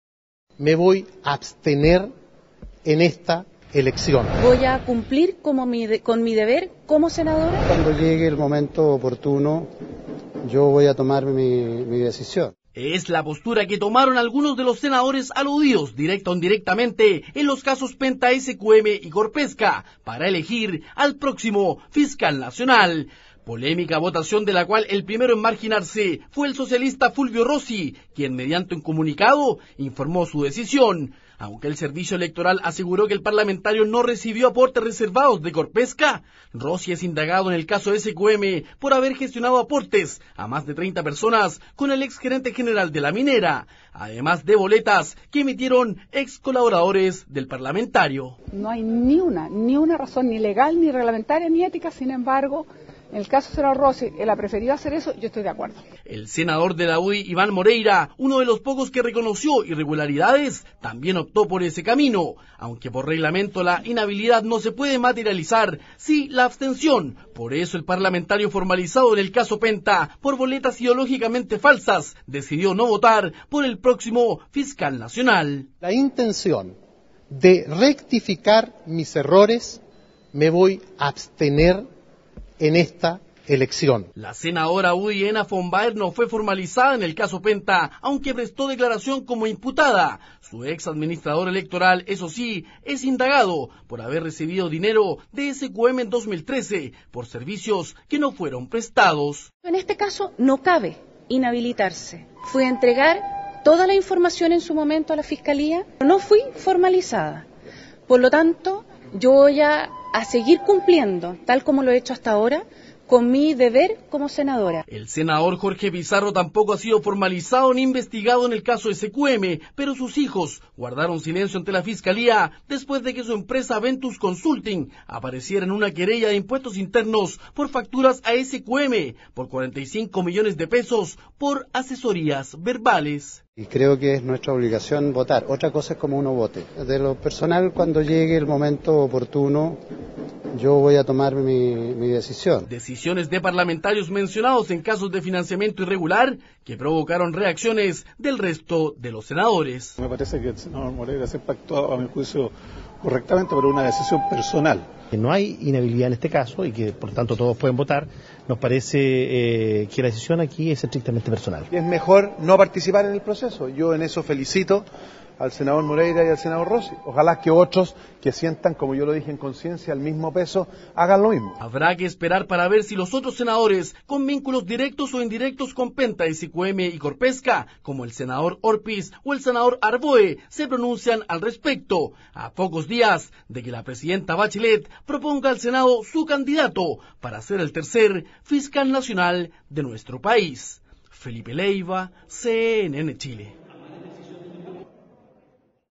Rossi y Moreira no elegirán a Fiscal Nacional
El senador socialista argumentó su decisión en "el clima de sospecha permanente" que se instaló en el país.